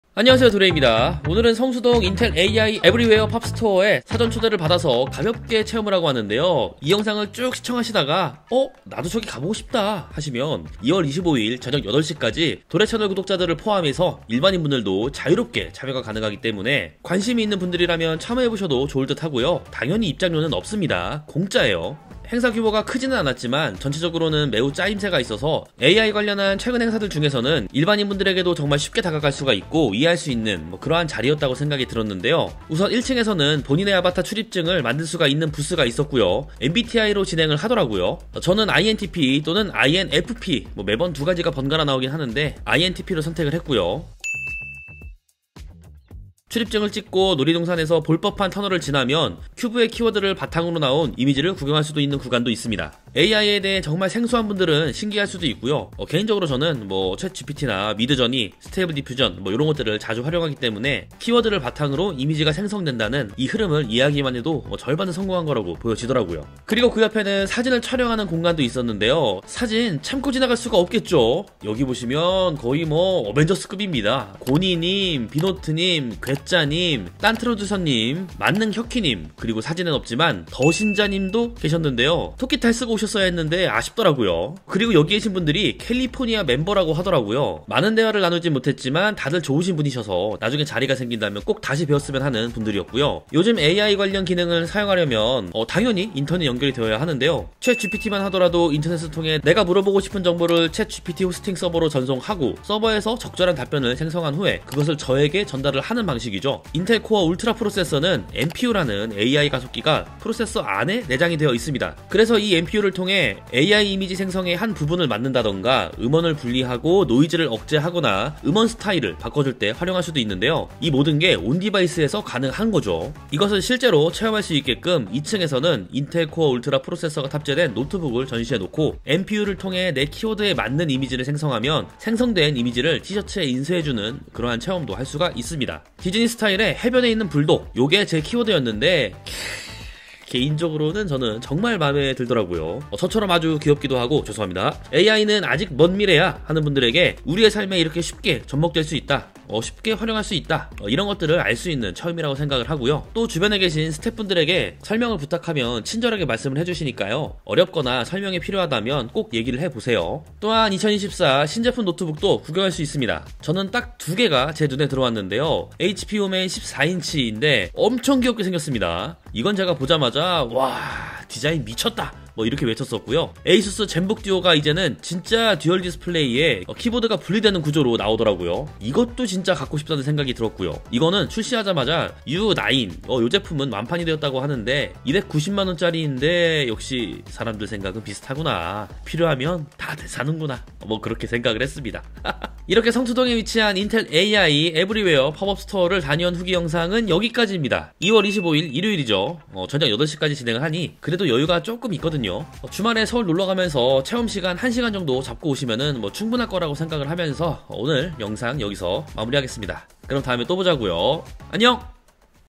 CPU?❌ GPU?❌ "NPU"로 AI 이미지를 생성해보았습니다!
인텔 AI Everywhere 팝업스토어 행사에 참여해봤습니다.
전문가 보다는 일반인 분들의 눈높이에 맞춰서 진행되어서
AI를 몰라도 쉽게 이해할 수 있다는 점이 좋았는데요.
꼭 한번 체험해보세요! 재미있더라고요 :)

📢 본 영상은 인텔 AI Everywhere 체험존을 참여한 영상입니다.

📌 위치: 플랜트란스 성수플래그쉽 (성수동 연무장11길 13)
📌 날짜: 2024년 2월 19일 ~ 25일 (12:00~20:00)

🕐 타임라인
0:00 성수동 인텔 AI 팝업스토어
0:33 AI 아바타 카드 & 큐브 생성 이미지
1:07 포토존
1:33 온디바이스 활용(NPU)
2:04 나만의 티셔츠 만들기
2:40 신제품 노트북 구경
3:13 마무리